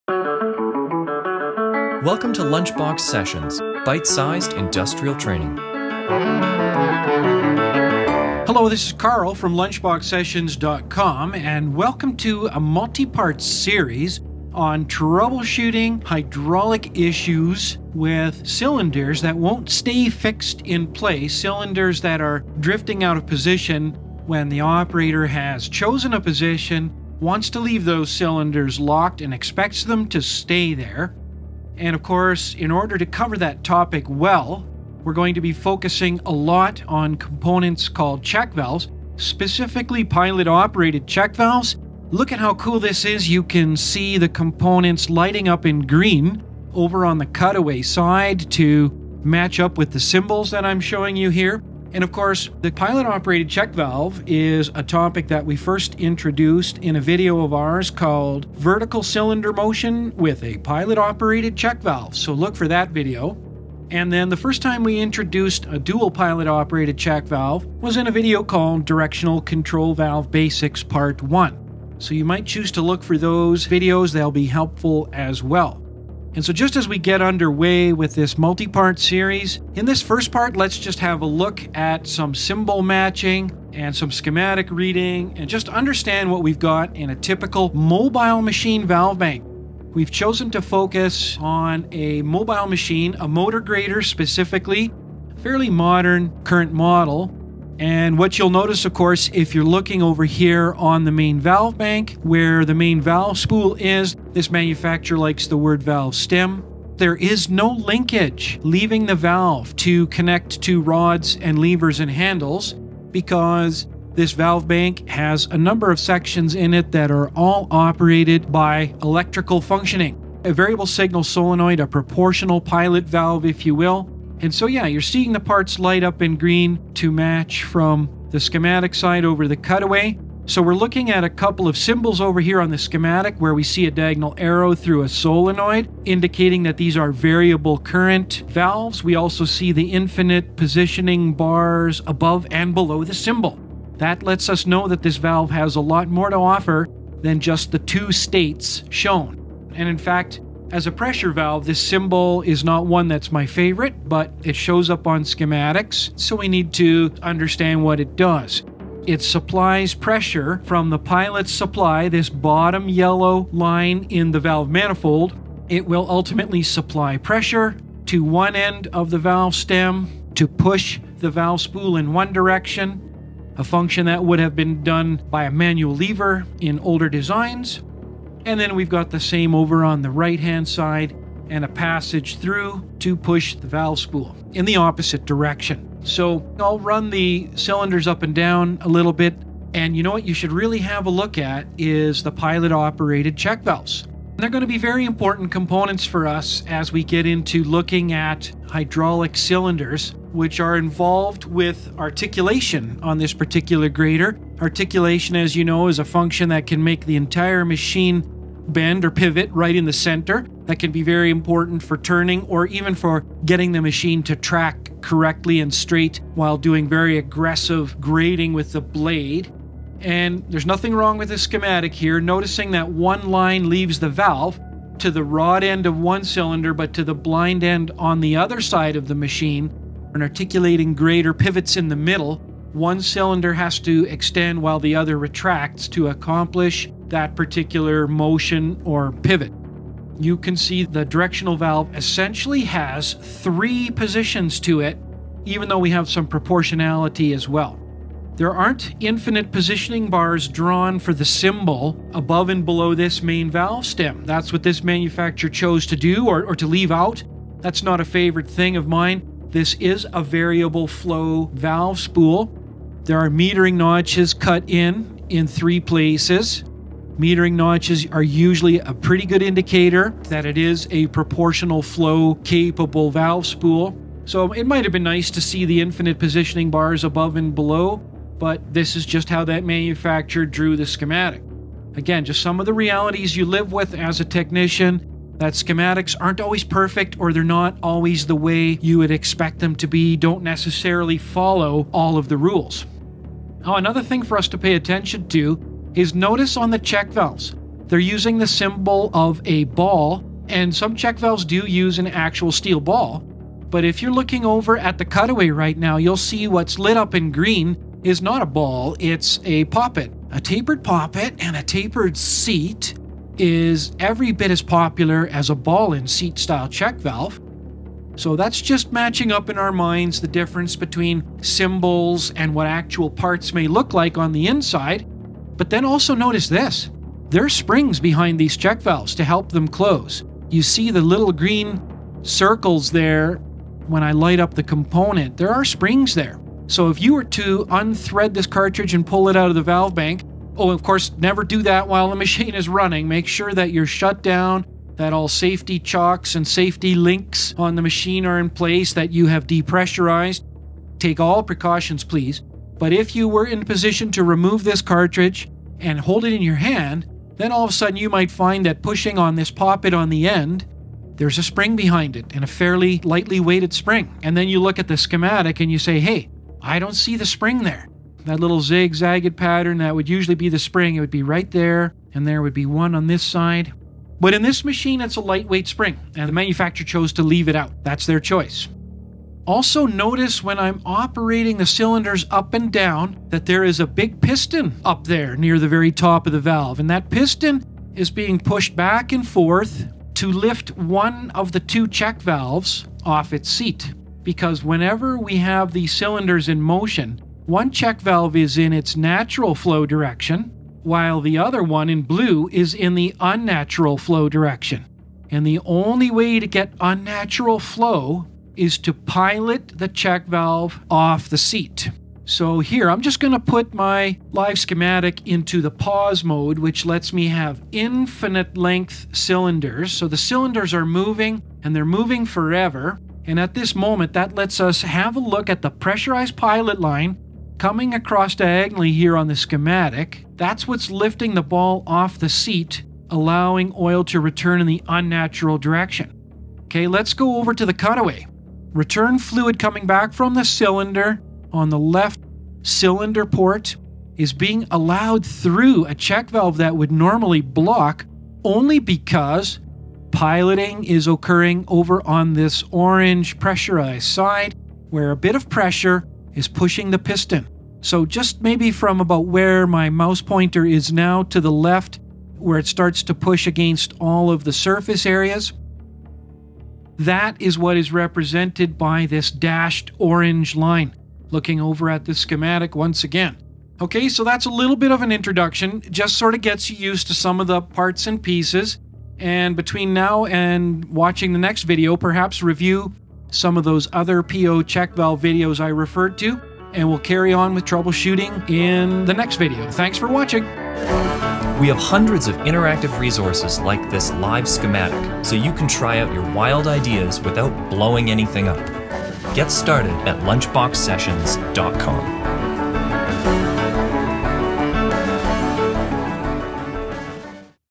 Pilot Operated Check Valve Cylinder Troubleshooting - Part 1
This video is the beginning of a series on troubleshooting hydraulic issues with cylinders that are drifting out of position. We'll start by looking at the internal details of our valve bank, focussing on the pilot operated check valves. You'll see how the manufacturer's schematic drawing matches up with the cutaway view of the valve.

These animations are fully interactive, and you can play with them yourself on our

Follow us for more great videos and learning content.